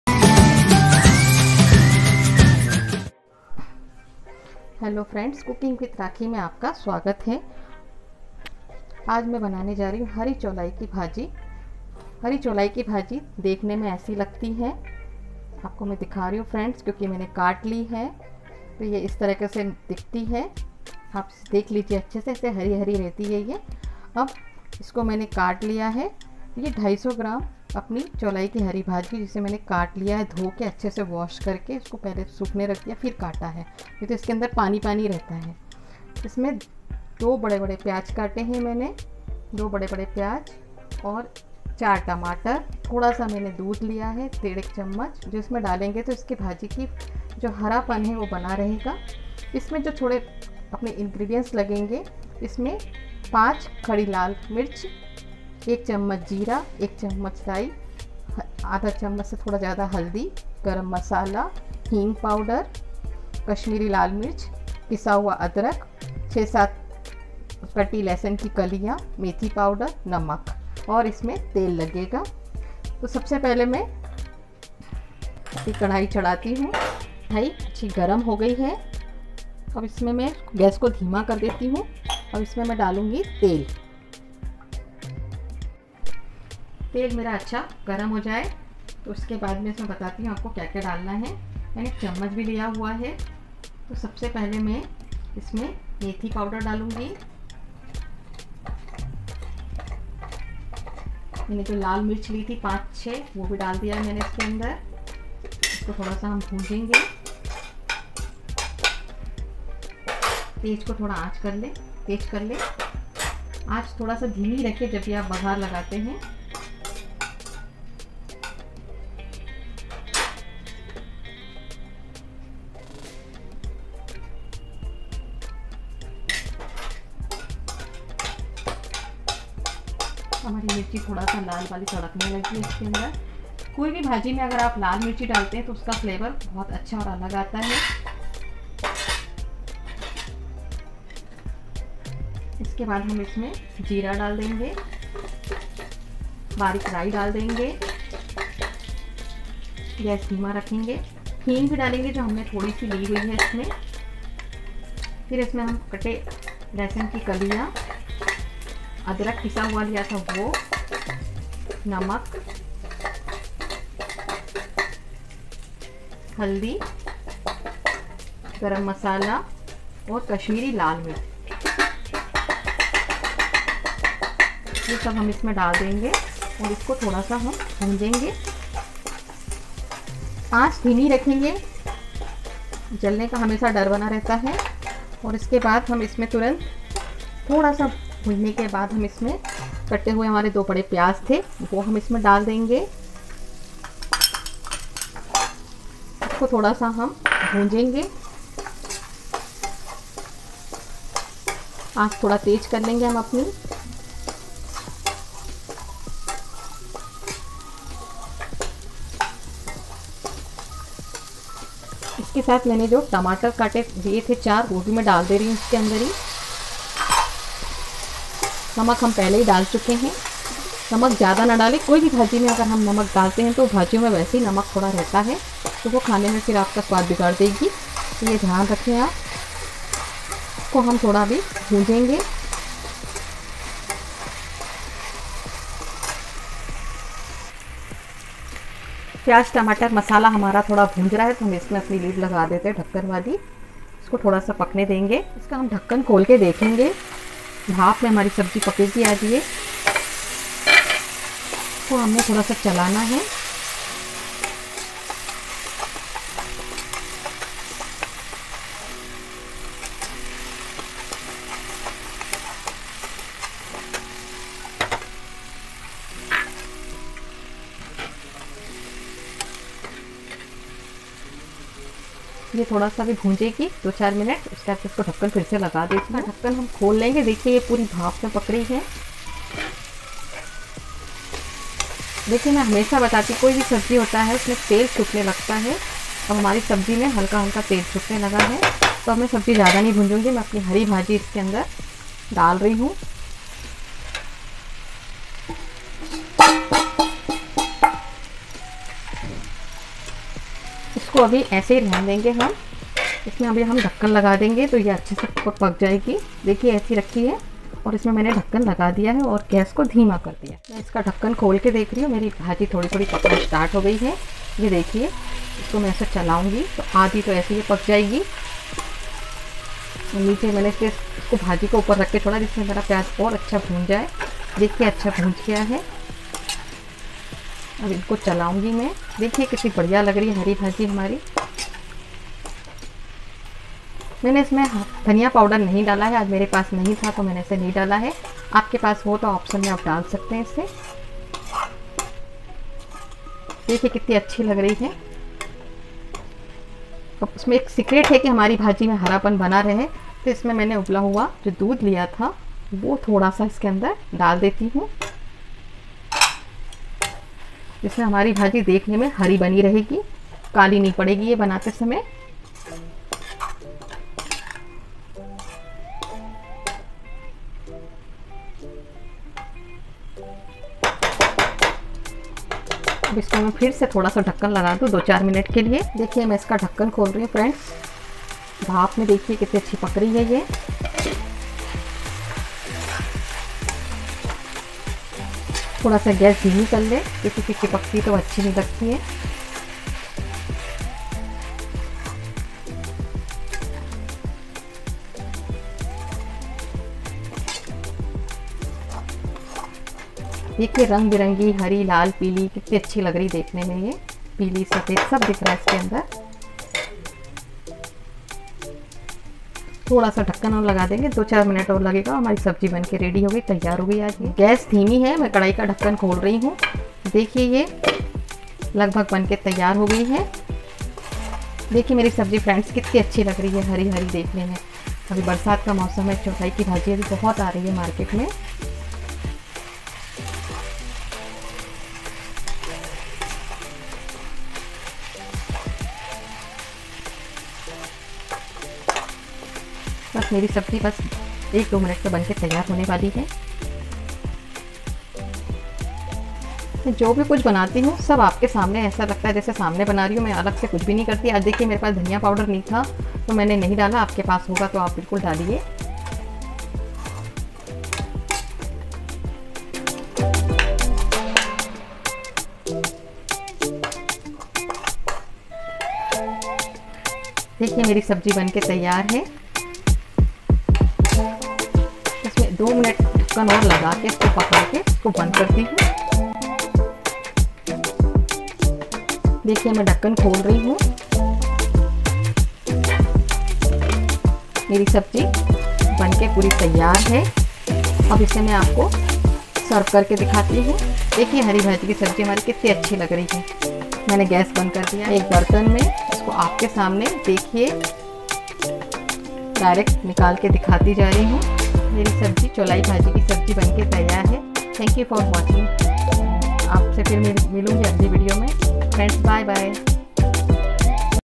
चौलाई की भाजी बनाने का तरीका | आसान तरीके से स्वादिष्ट और मजेदार सब्जी बनाए
Cholai की bhaji in easy steps